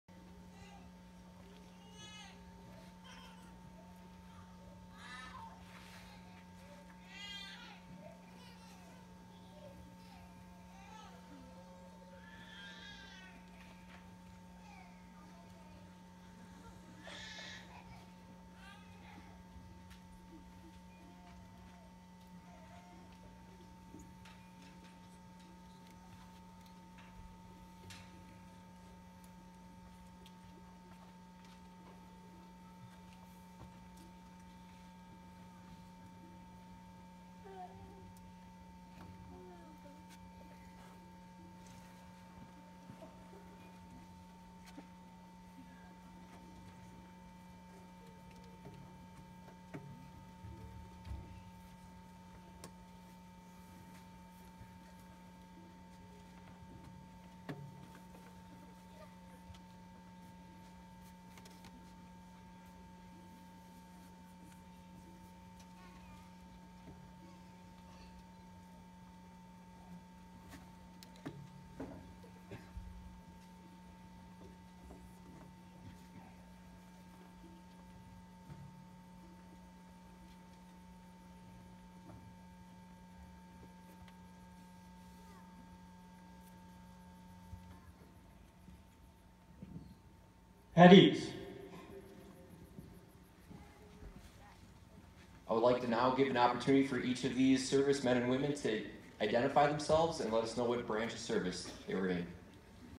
Mount Rushmore
The lowering and folding of the flag at Mt. Rushmore in front of all the veterans. July 2016